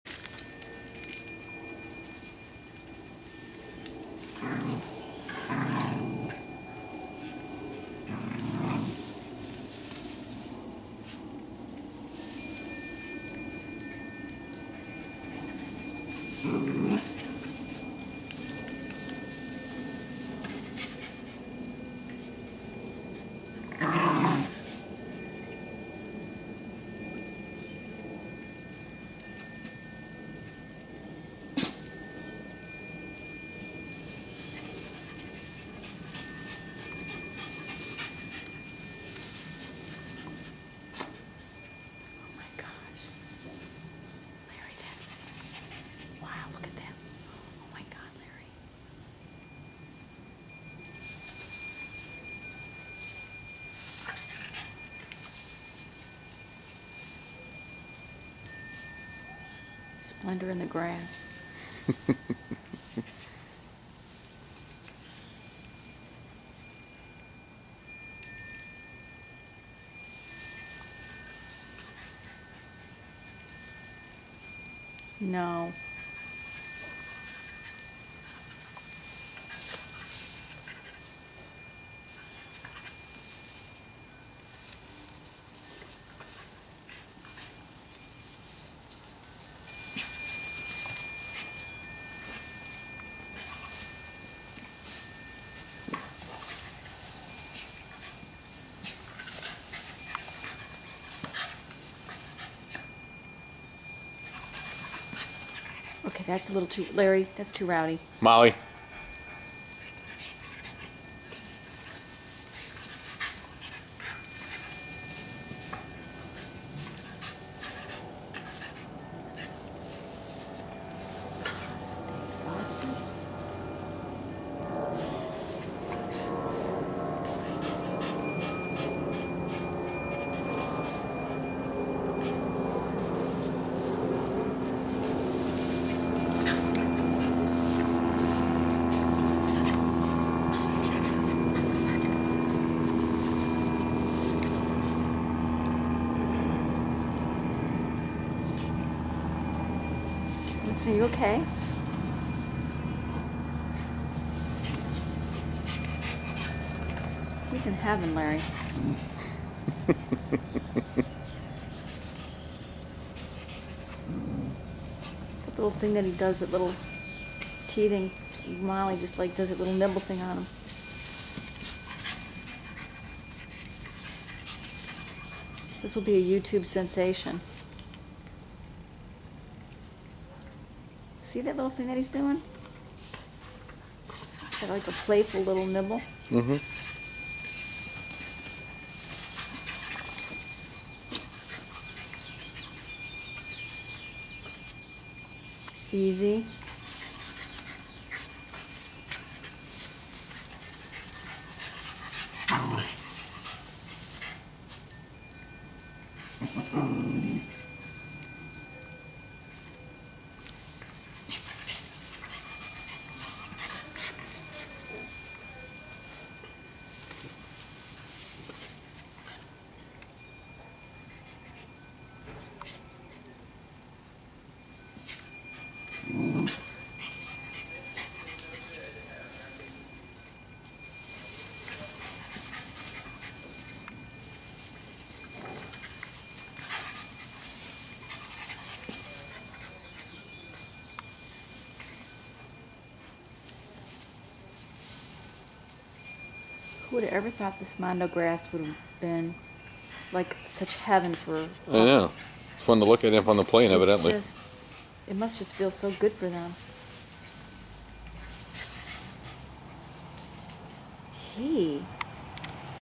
Terrier Tussle
Beautiful and tenacious Norwich Terrier puppy named Winston has a good time playing with his visiting half sister, Molly, a Jack Russell Terrier mix.